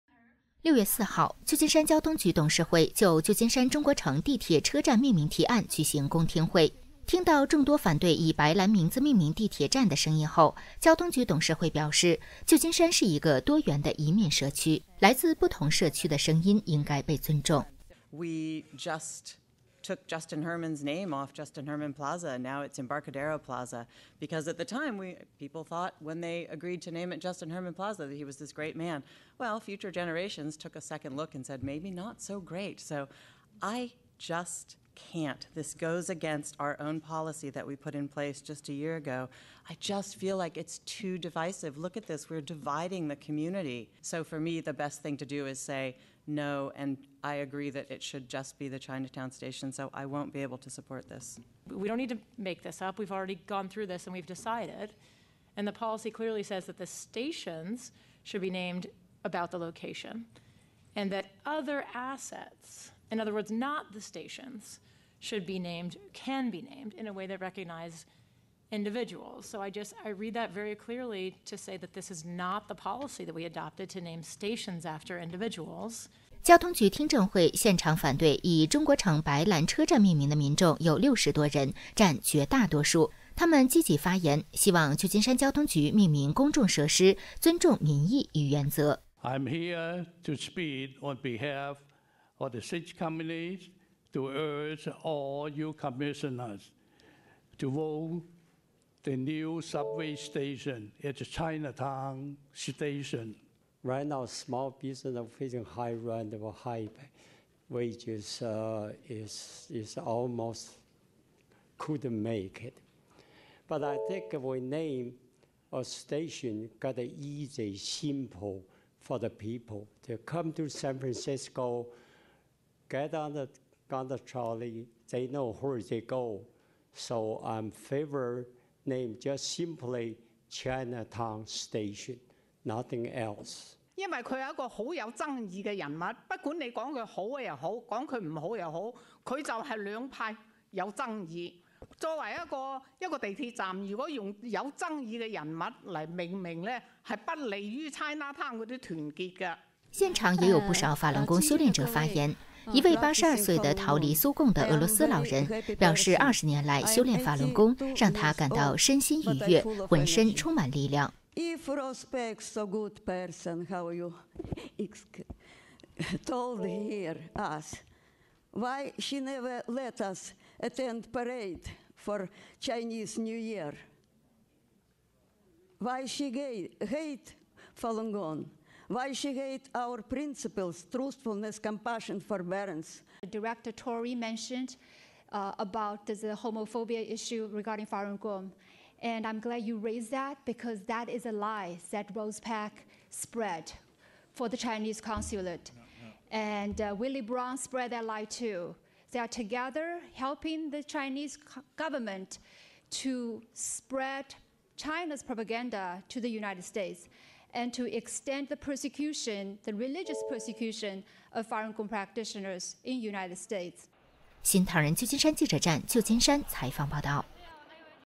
「白蘭站」命名被擱置 交通局質疑提案撕裂社區
6月4號，舊金山交通局董事會和舊金山市議會討論就舊金山中央地鐵華埠站命名提案分別討論。雖然舊金山市議會通過了敦促交通局，在地鐵站名字中加入「白蘭」的人名。不過，交通局董事會經過三小時的公聽會後，最終延後動議以3：3，維持交通局原有「不以人名，而以地名命名車站」的既定政策，擱置以「中國城白蘭站」命名即將開通的中國城地鐵站。